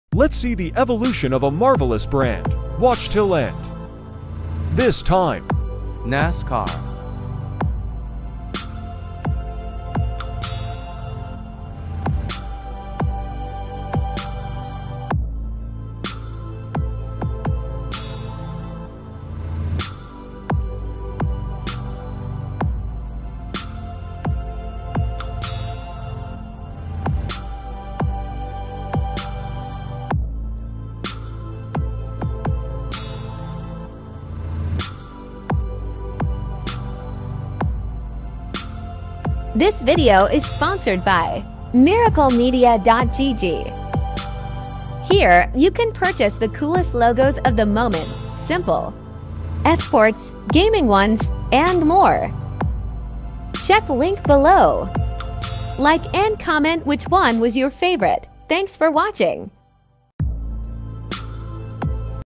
NASCAR LOGO EVOLUTION
The National Association for Stock Car Auto Racing, LLC (NASCAR) is an American auto racing sanctioning and operating company that is best known for stock car racing. It is considered to be one of the top ranked motorsports organizations in the world, and is one of the largest spectator sports in America.